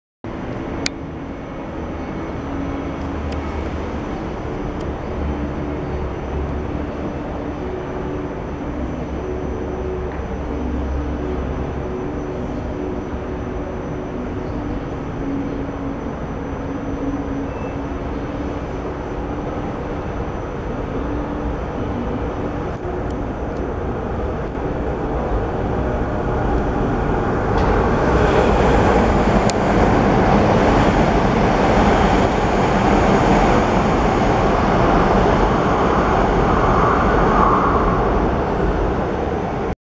機場捷運直達車 往第二航廈 通過A5泰山站
錄於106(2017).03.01 PM03:27，A5泰山站，本列車往機場第二航廈站，等車時拍攝。